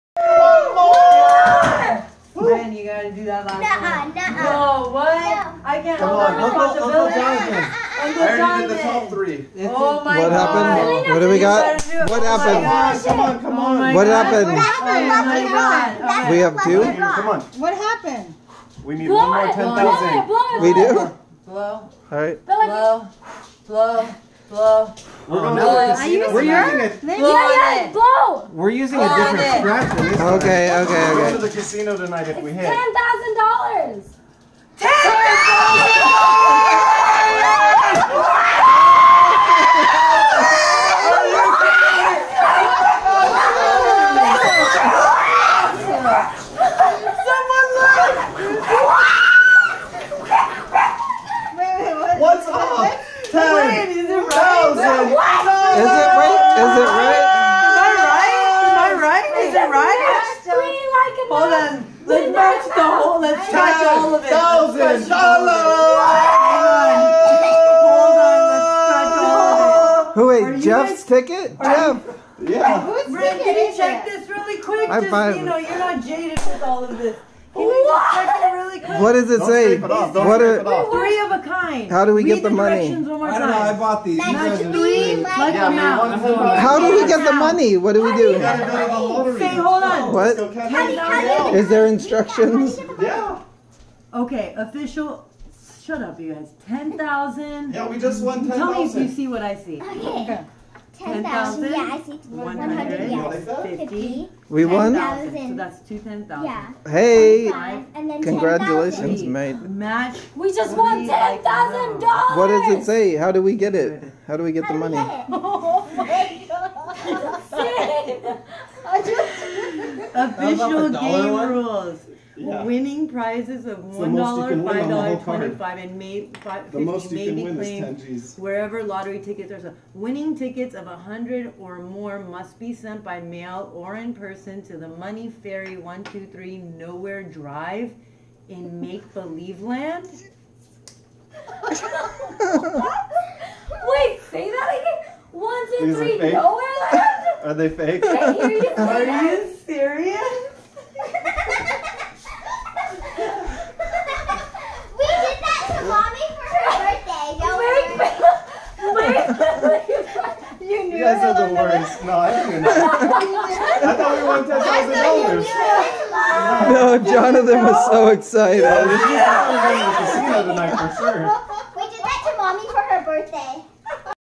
Fake lottery ticket brother. Only the two scratchers didn't
My brother and his wife didn't know everyone else was aware.